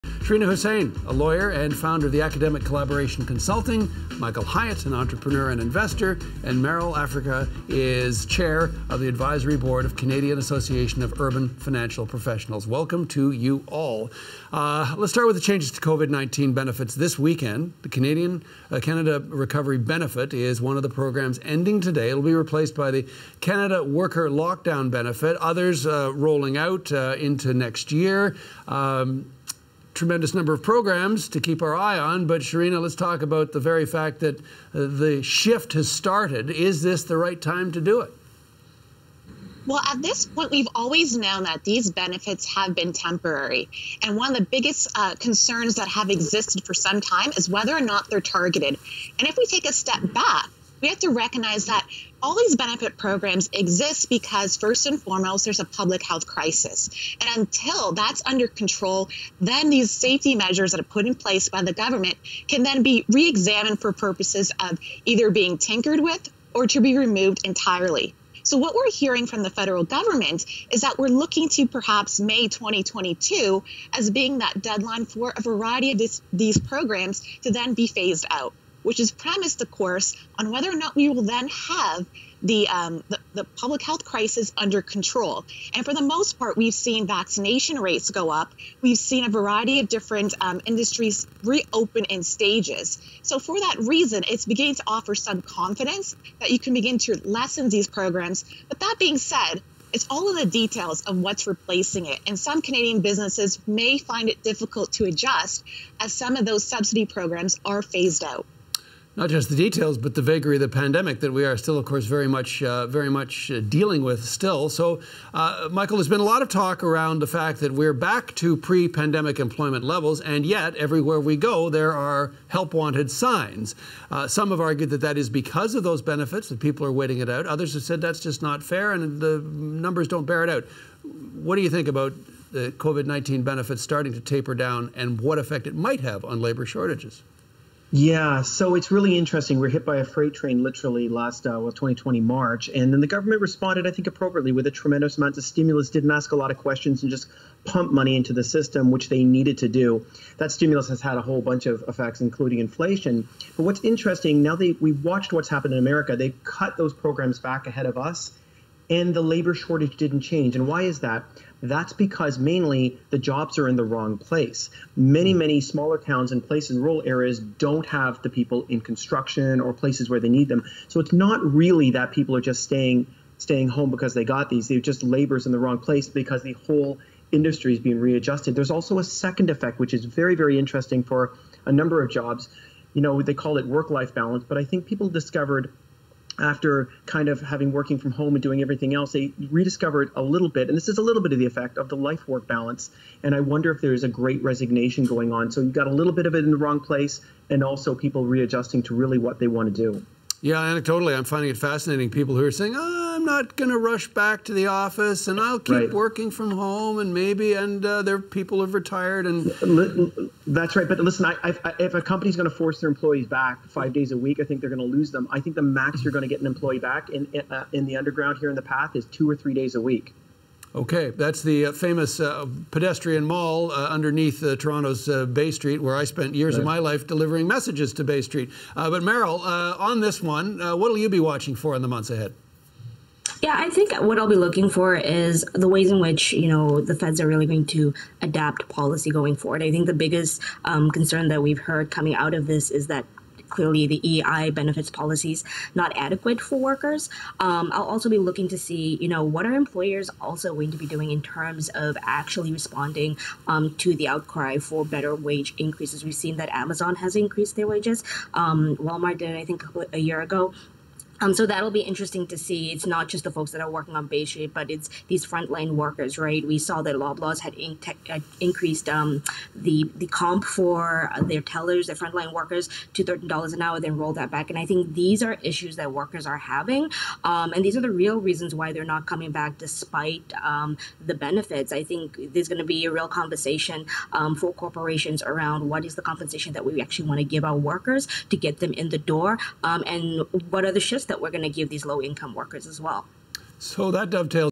Pandemic benefits expire and new programs to begin
The Liberal government says several existing pandemic support programs for individuals and businesses will end Saturday, while billions of dollars will be spent transitioning to new 'targeted measures' leading into the spring.